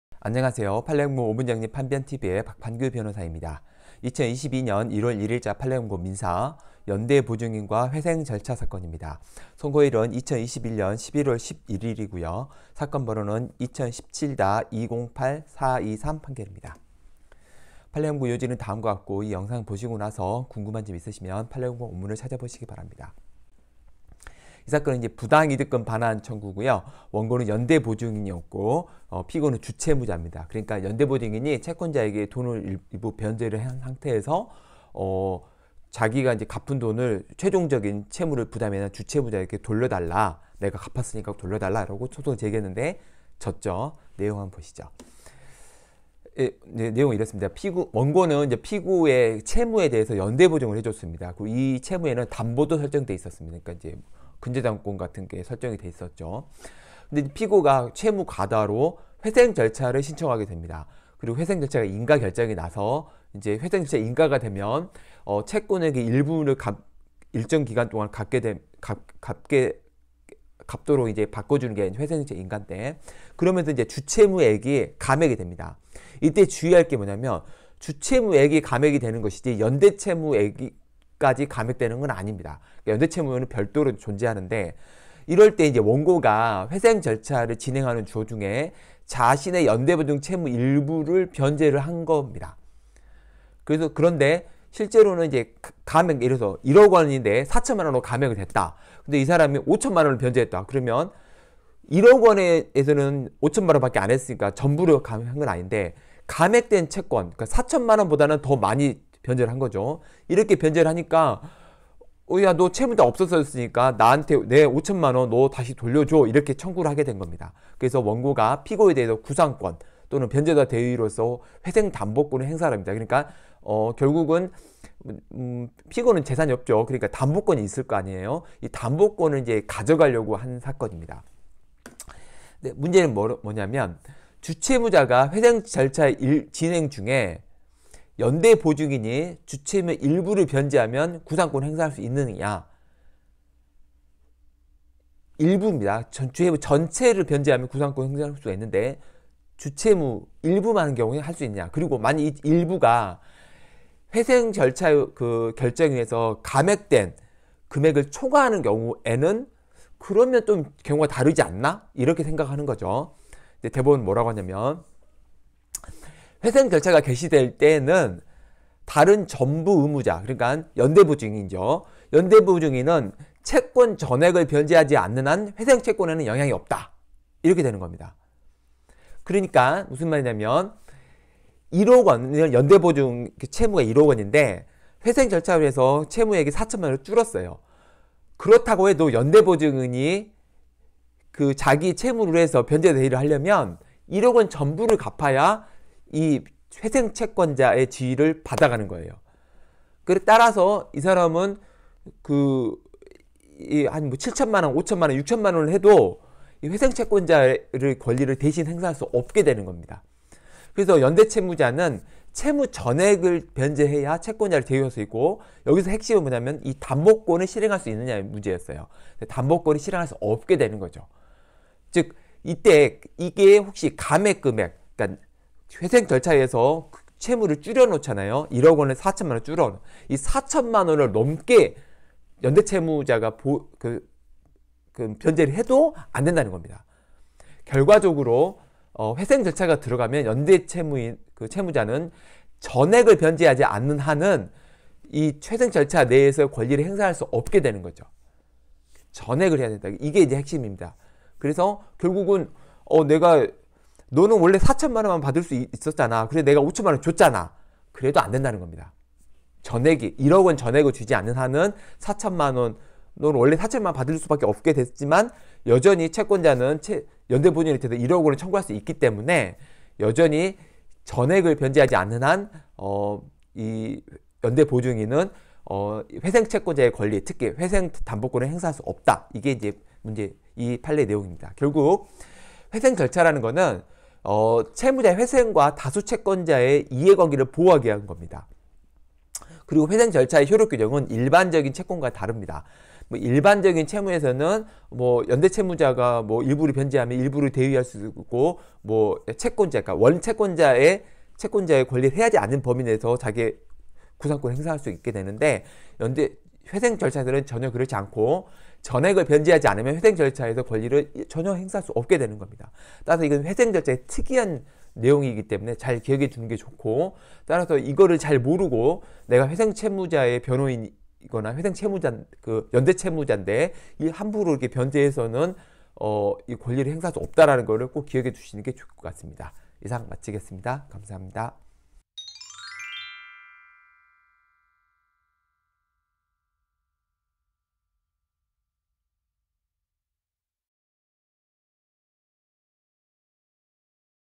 연대보증인과 회생절차 사건 [22.1.1.자 판례공보(민사)
연대보증인과 회생절차 사건
별 3개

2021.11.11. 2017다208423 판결
부당이득금반환

원고 연대보증인(상고인)
피고 주채무자

상고기각

원고는 피고의 채무에 대하여 연대보증함, 이 채무에는 담보도 설정되었음
피고는 채무과다로 회생절차 신청, 인가결정됨
인가결정으로 주채무가 감액됨
원고가 회생절차 진행 중 자신의 연대보증 채무 일부(감액 금액은 초과)를 변제함
원고는 피고에 대하여 구상권 내지 변제자대위로서 회생담보권을 행사하려 함

주채무자가 회생절차 진행 중
연대보증인이 주채무의 일부를 변제하면 구상권행사할 수 있나?
감액 금액은 초과하였다면 다른가?

회생절차 개시시에는 다른 전부의무자가 채권 전액을 변제하지 않는 한 회생채권액에는 영향 없음
연대채무자는 채무 전액을 변제해야 채권자를 대위할 수 있음
감액 금액을 초과하여 변제하더라도 전액 변제가 아니면 대위할 수 없음

실무활용
회생절차는 채무자 회생과 다수 채권자들을 보호하기 위한 것
회생절차에서의 효력 규정은 일반 채권과는 다름
연대채무자의 대위(구상권, 담보취득)은 전액 변제에만 가능함
회생절차의 특이한 부분은 잘 기억해 두어야 함